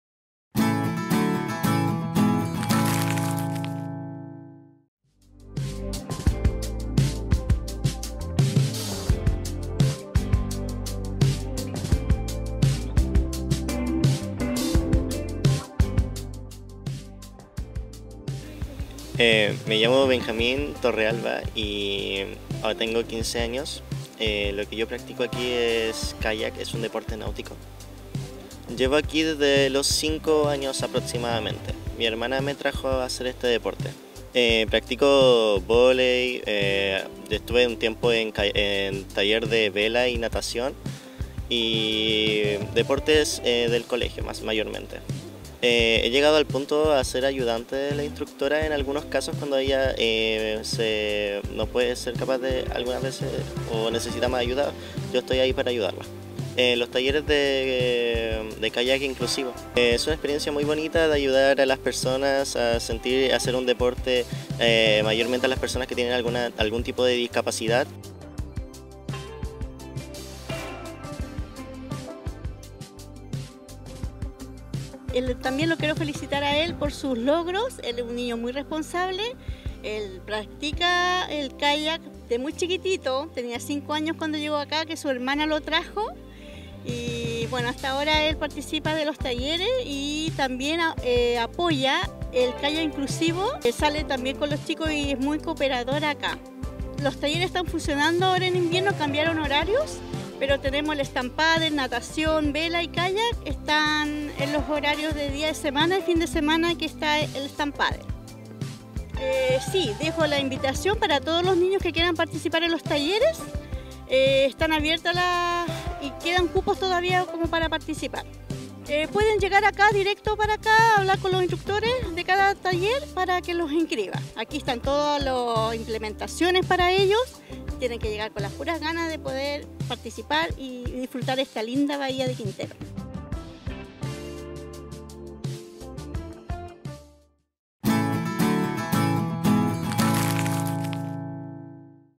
Deportes Acuáticos En Cendyr Náutico
El Cendyr Náutico alberga una variedad de deportes acuáticos entre los que destacan la vela, el stand up paddle y el kayak.

Benjamín Torrealba lleva cerca de 10 años practicando este último deporte y se convirtió en un ejemplo al ayudar a las personas en situación de discapacidad que participan del kayak inclusivo.